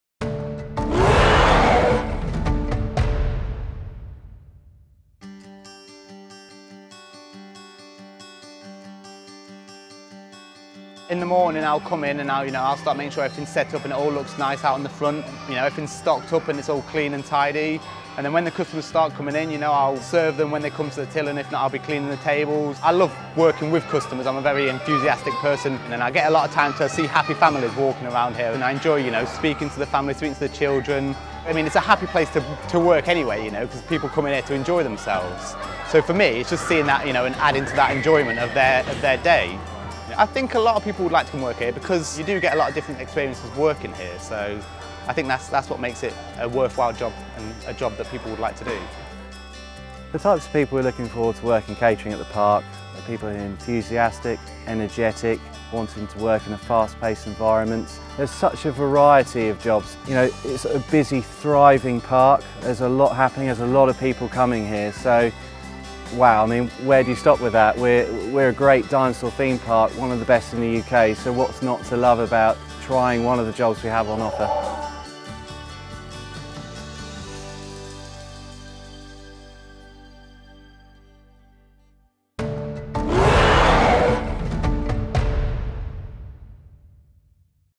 Meet the Crew - Catering
Recruitment video promoting the catering staff roles at Dinosaur Adventure.